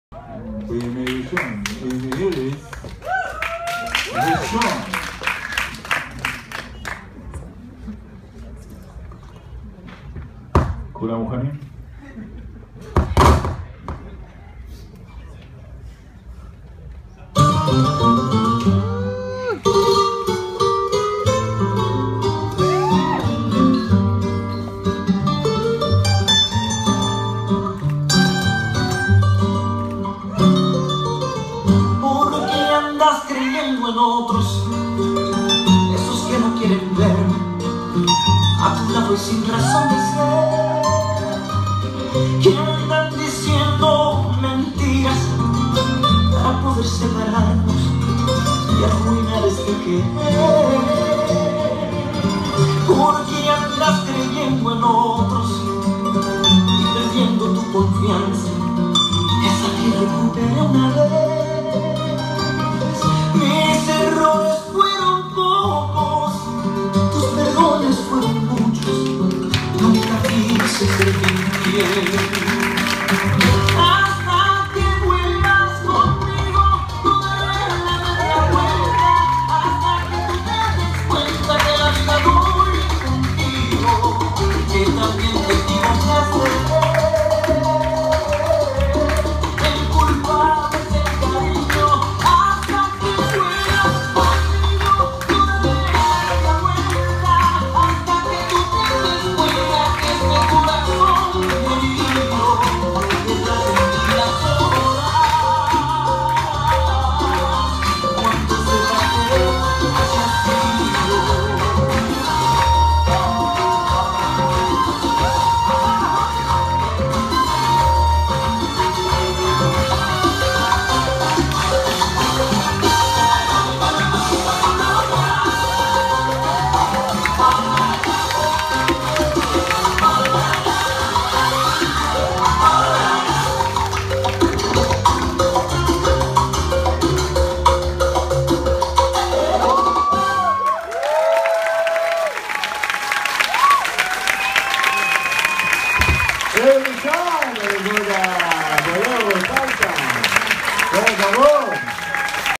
איריס ושון בריקוד בולרו וסלסה!
איריס גולדשטיין ושון זיו בריקוד שמשלב בין בולרו לסלסה!
בבית הפתוח של חודש יולי בסטודיו דאנס תל אביב!
איך למצוא אותנו:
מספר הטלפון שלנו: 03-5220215
דיזנגוף 98, תל אביב